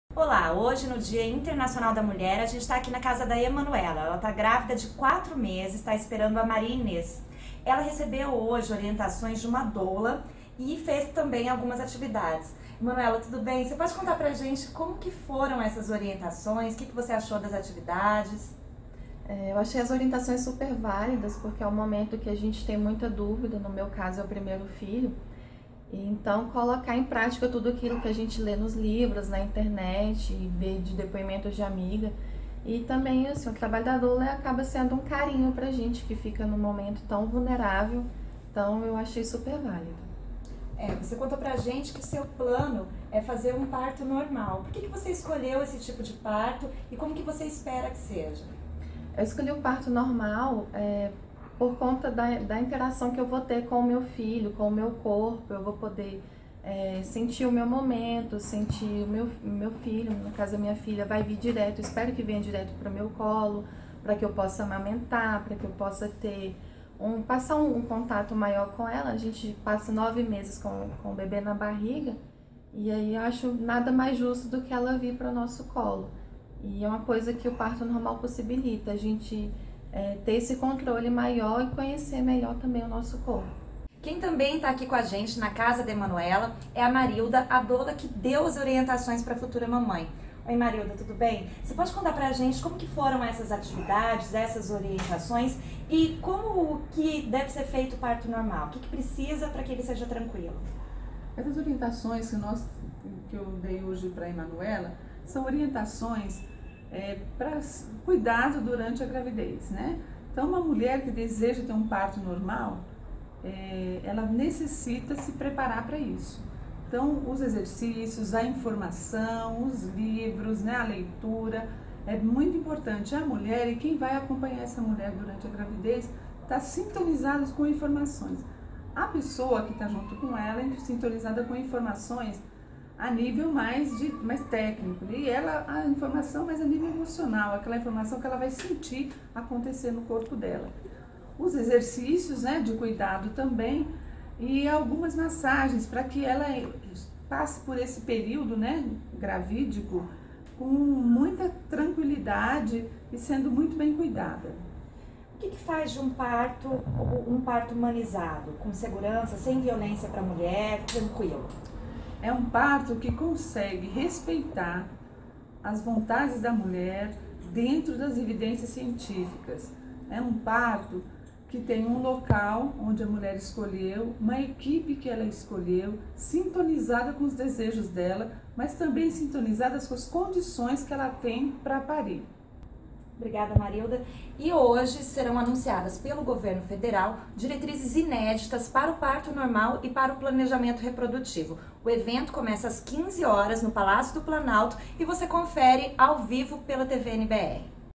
Conheça o trabalho das mulheres que apoiam as gestantes durante o parto humanizado
ESPECIAL WEB - 08.03.17: Conheça o trabalho das doulas, mulheres que ajudam e apoiam gestantes durante a gravidez e na hora do parto. O trabalho feito pelas doulas proporciona um parto humanizado e pode reduzir a violência obstétrica. Para celebrar o Dia Internacional da Mulher, o governo federal vai apresentar, logo mais, diretrizes inéditas relativas ao parto normal e ao planejamento reprodutivo.